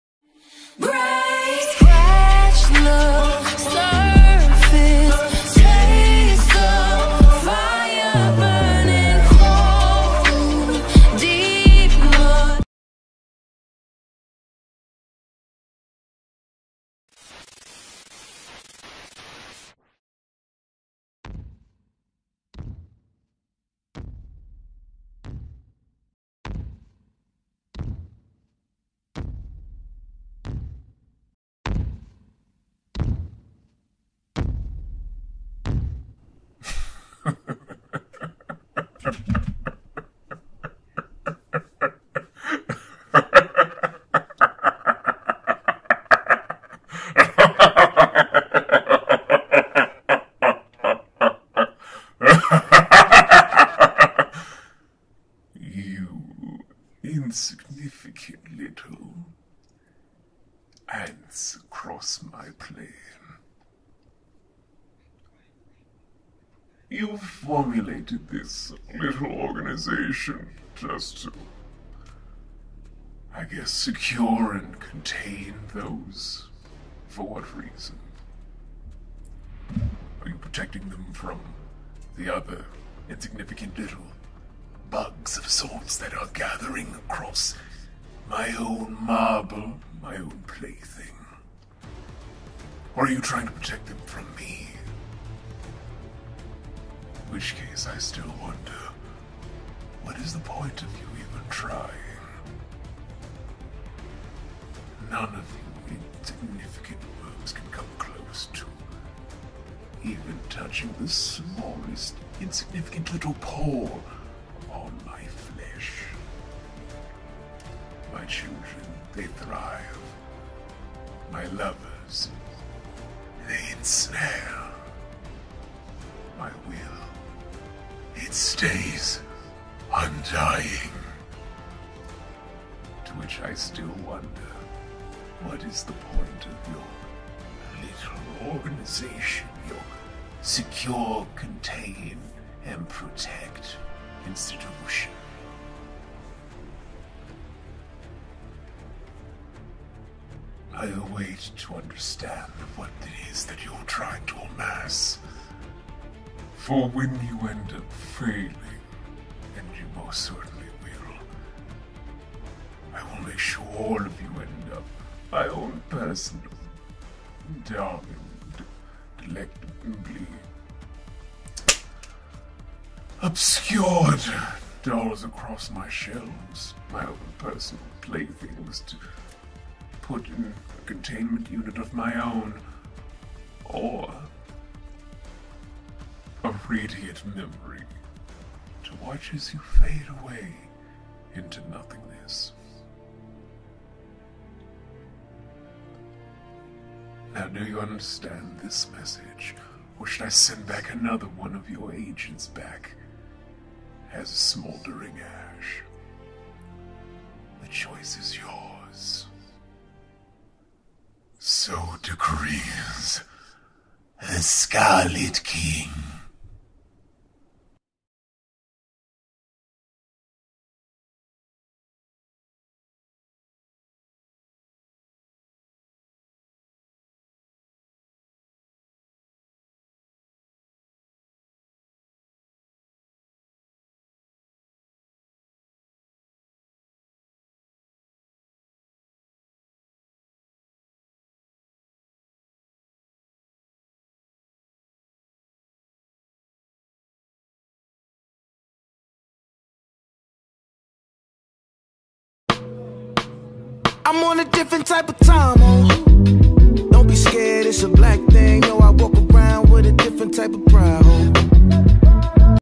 Voice Acting Attempt: SCP The Scarlet King - Round 1
In this new installment of "Voice-Acting Attempt", Vi is going back into the world of SCPs and tackling one of the Keter Class creatures.
Which one will it be? What voice did he give it? Will it be one of Joy's Siblings??
Let's see what he has conjured up for this 1st Round installments.

Scripted by: Phineas
Edited by: Nick T
Sound Mixed by: Vi

Next Voice-Acting Attempt (For Real This Time): Critical Role Character